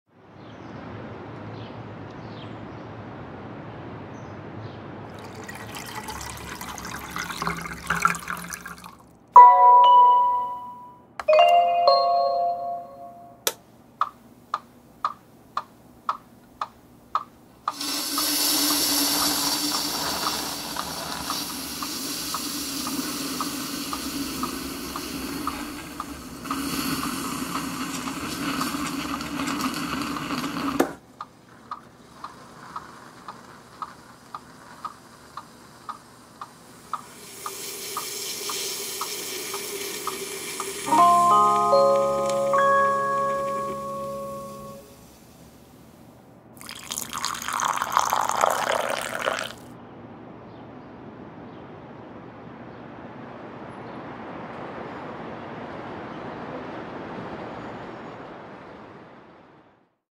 【ビックカメラ】バルミューダ提供 ドリップ式コーヒーメーカー「BALMUDA The Brew」を動画でご紹介
バルミューダ提供 ドリップ式コーヒーメーカー「BALMUDA The Brew」を動画でご案内いたします。